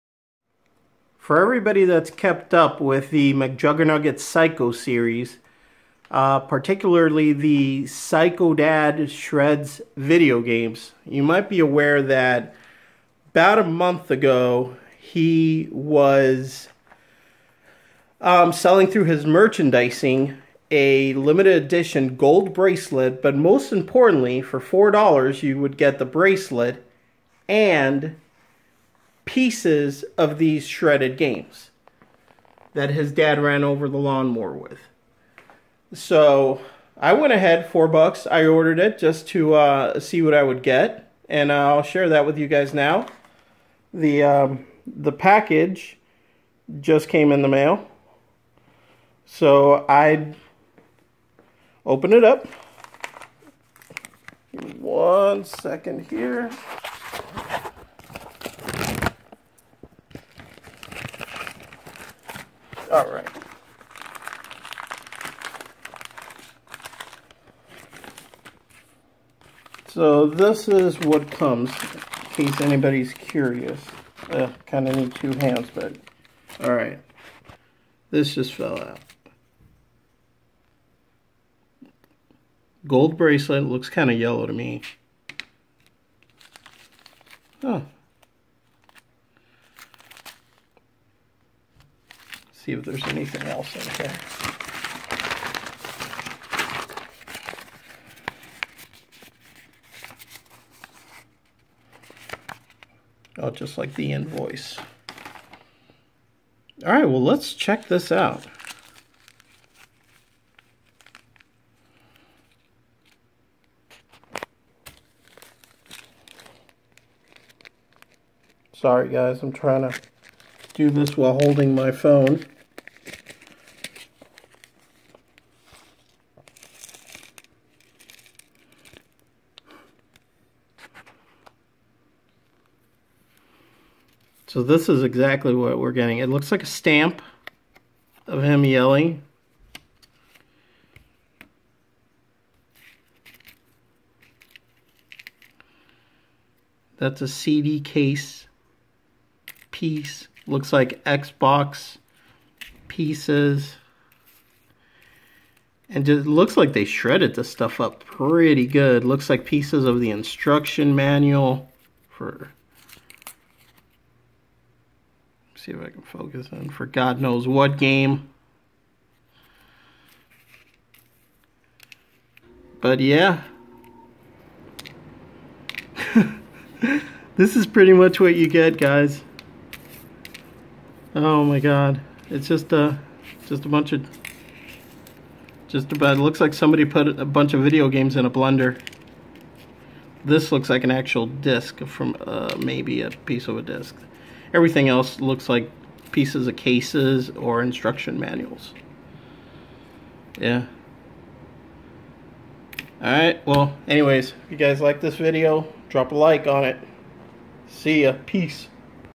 Pieces from Mcjuggernuggets!!
This is the opening of the mcjuggernuggets gold bracelet and shredded game pieces. Limited edition